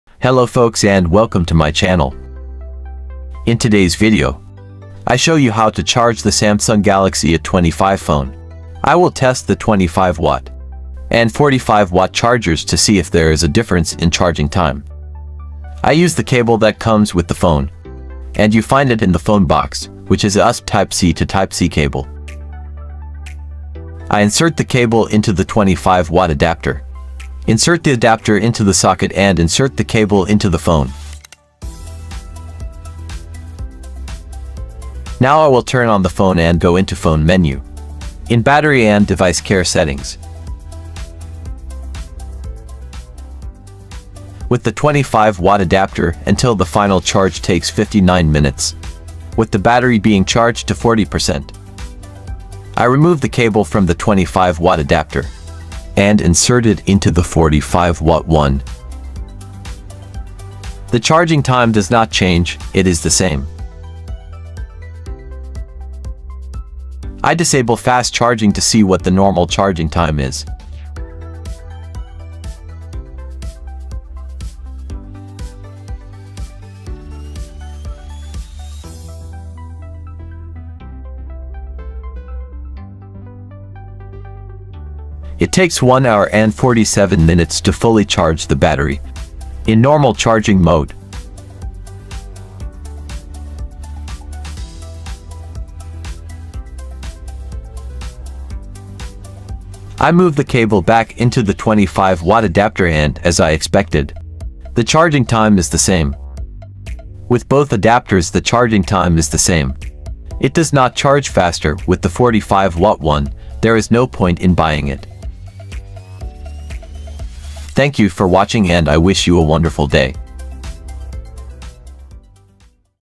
Samsung Galaxy A25 charging with 25 and 45 watt chargers
In this video I test the original 25 and 45 watt Samsung chargers to see which one charges the Samsung Galaxy A25 faster .